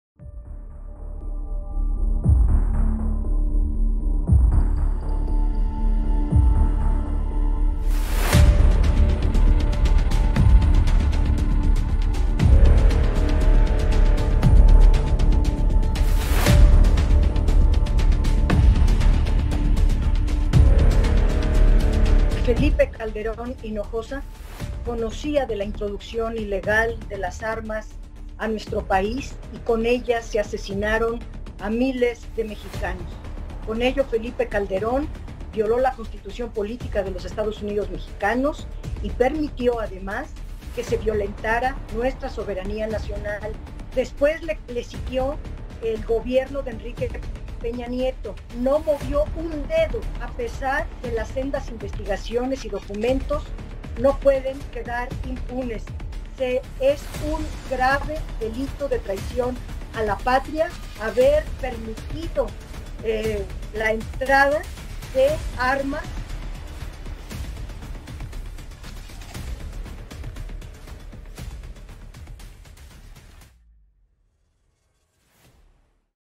¡El Operativo Rápido y Furioso Cobró La Vida de Miles de Mexicanos! ¡No Lo Vamos a Dejar Pasar!
El operativo Rápido y Furioso orquestado en EE.UU y que Calderon permitió cobró la vida de miles de mexicanos.  ¡Se violentó la soberanía nacional y no lo vamos a dejar pasar!
Dip. Dolores Padierna